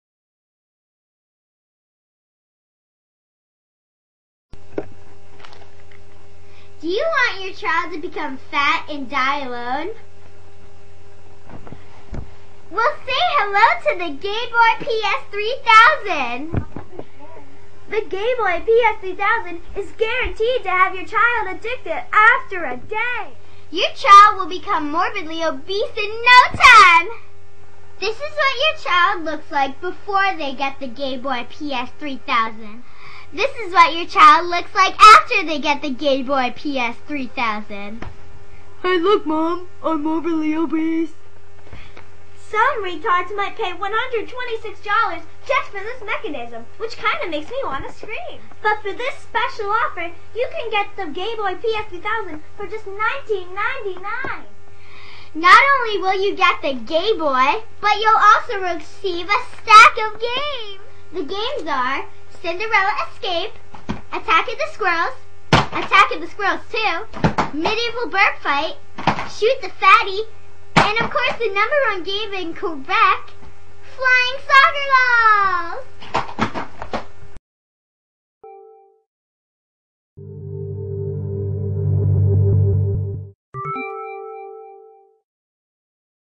The Gay Boy P.S 3000
It's a parody of the game boy but were not trying to make fun of nintendo or obese people we just wanted to make a funny video. Thnx!